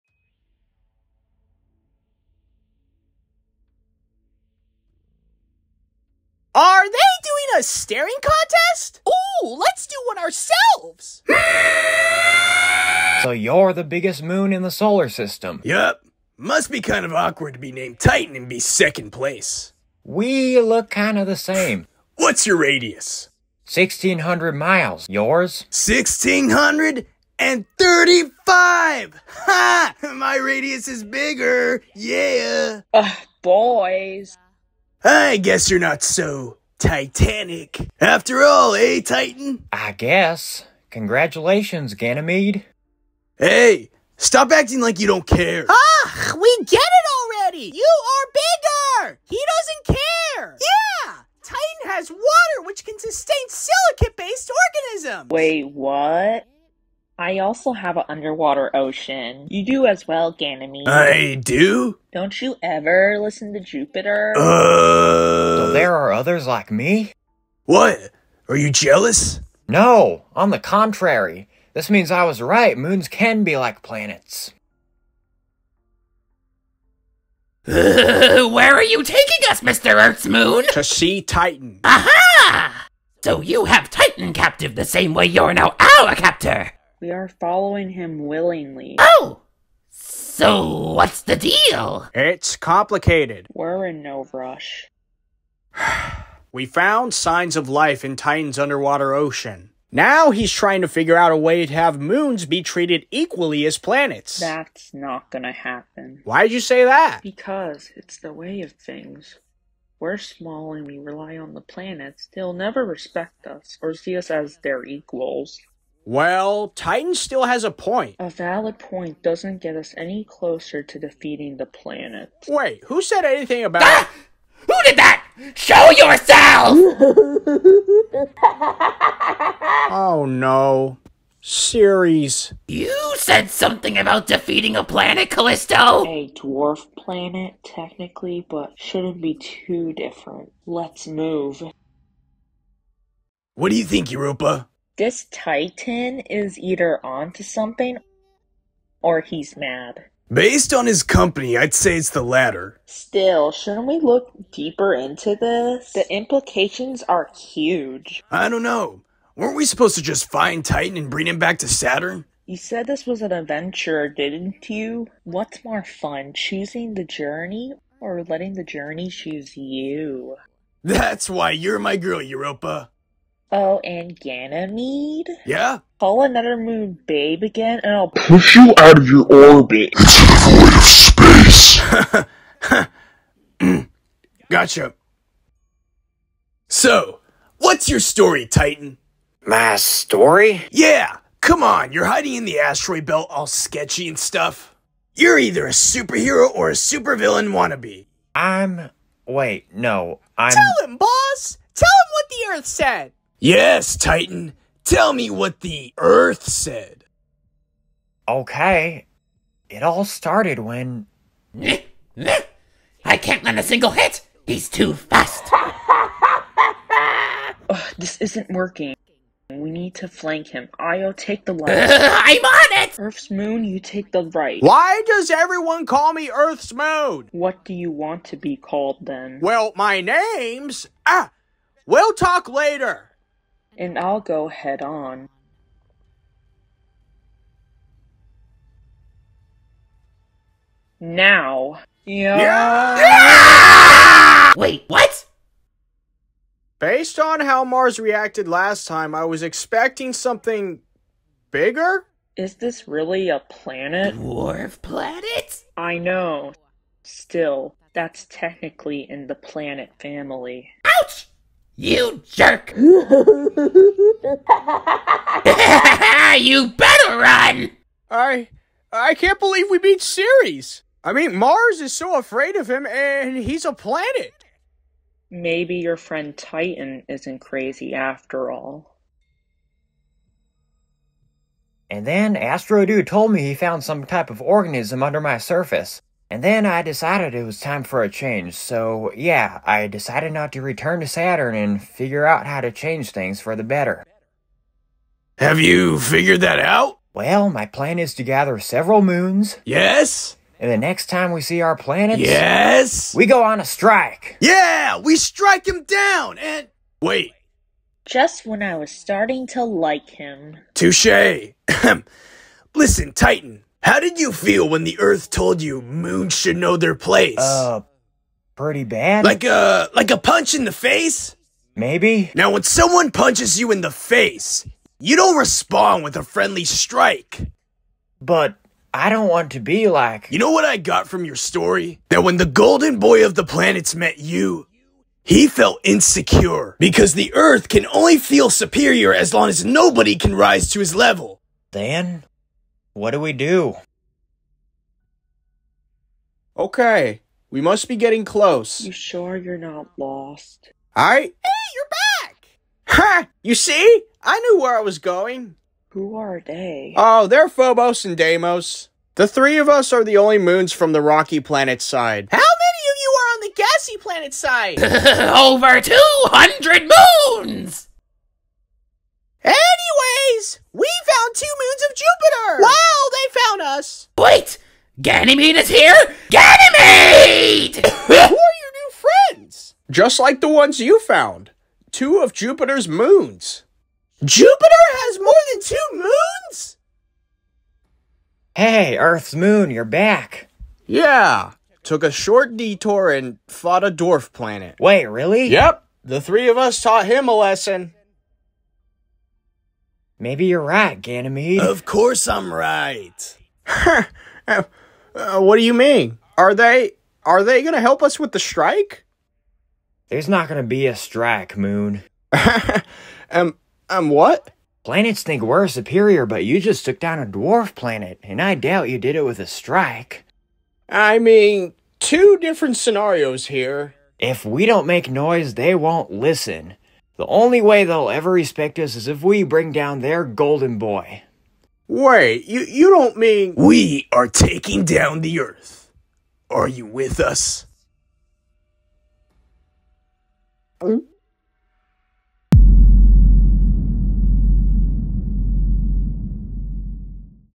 The Moons of Jupiter - Part 2 (My Voice)
solarballs spaceball spaceballs Welcome to the final video of 2023! It's been an honor rolling with you guys this year on my channel and I'm excited for what 2024 will bring!

Europa, and Callisto Voice Actress: @Lindy_14_

IO Voice Actor: @CruzingAroundTheBlock

Titan Voice Actor: @UranusIsHere7

Ceres Voice Actor: @Hooda2_0